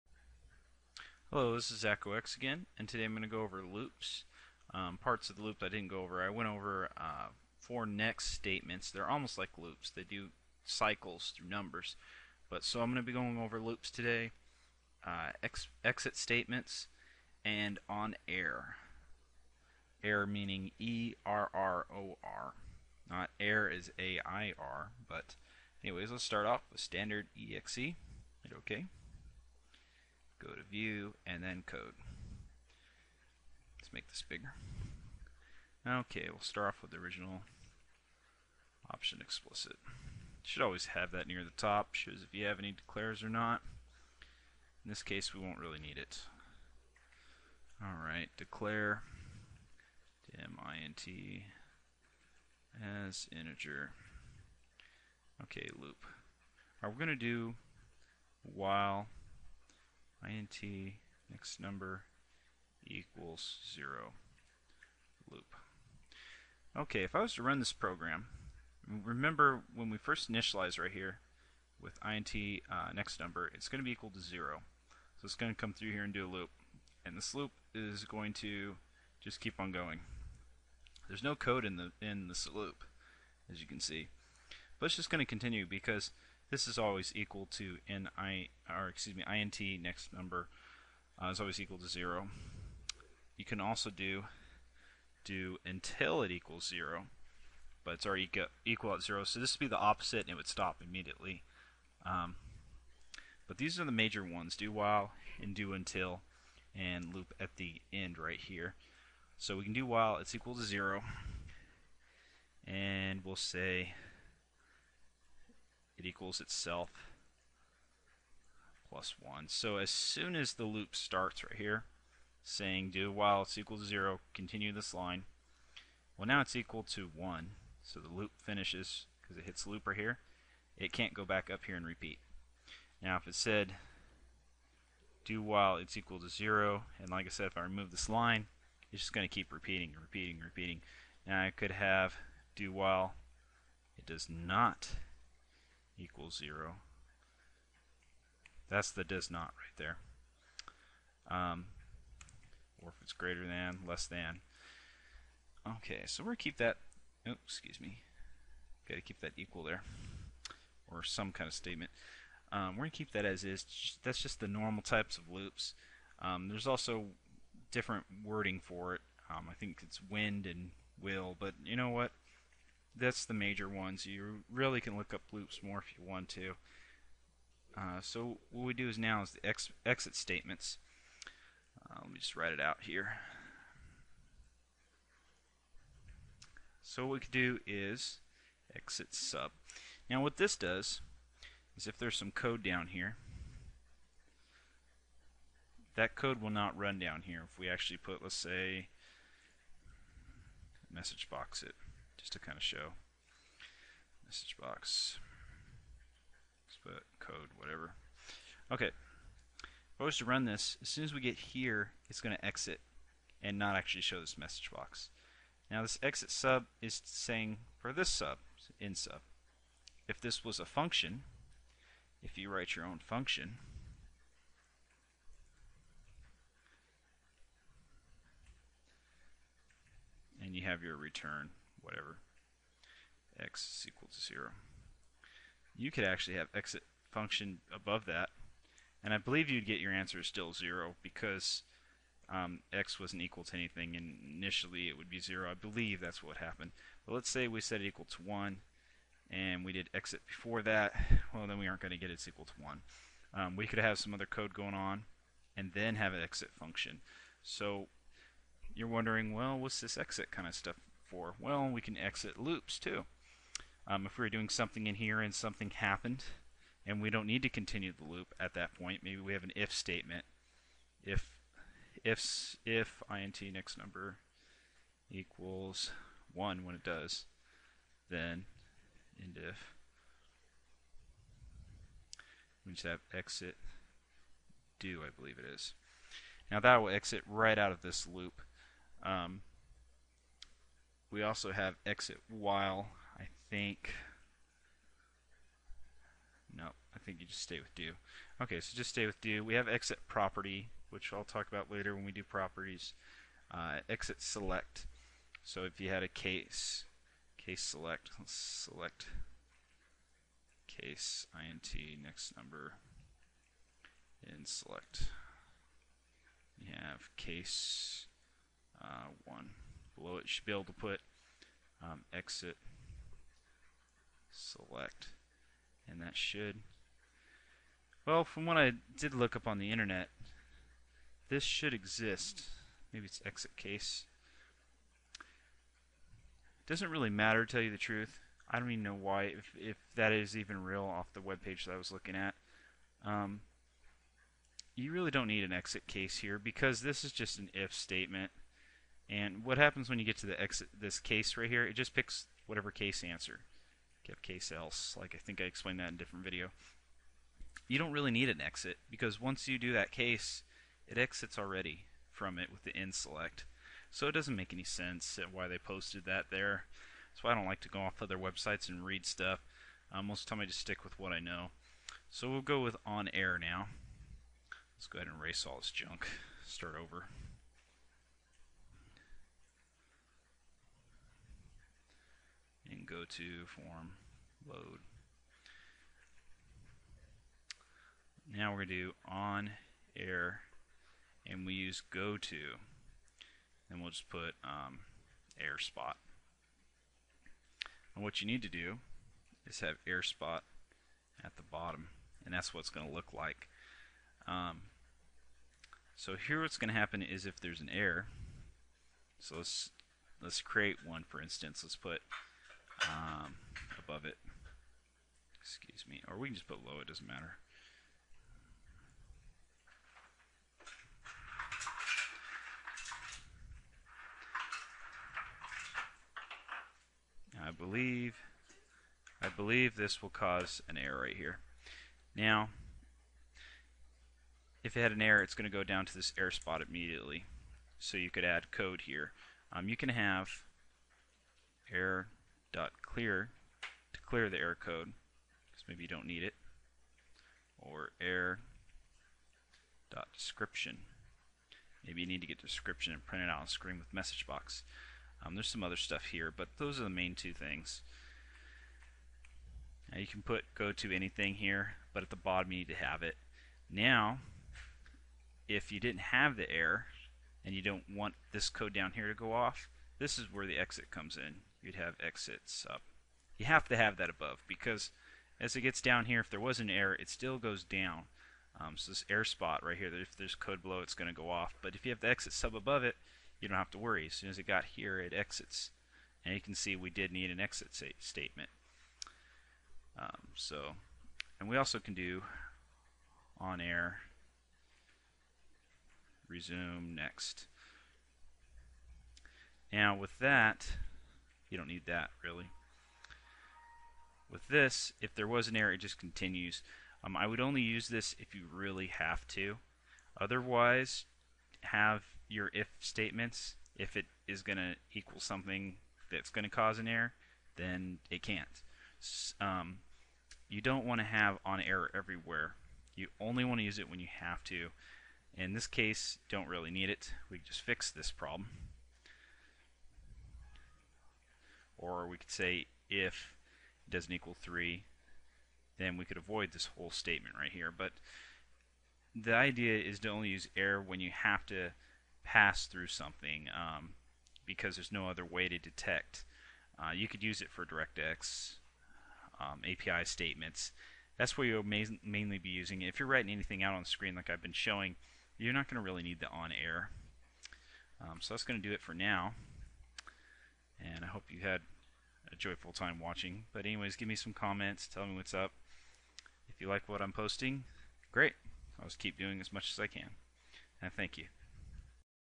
Video 16: VB 6 Programming, Loops, Exit Statements, On Error
Do While, Do Until
Loop

Exit Statements:
Exit Sub
Exit Function
Exit Do
Exit Property
Exit For

On Error Goto Word
Word: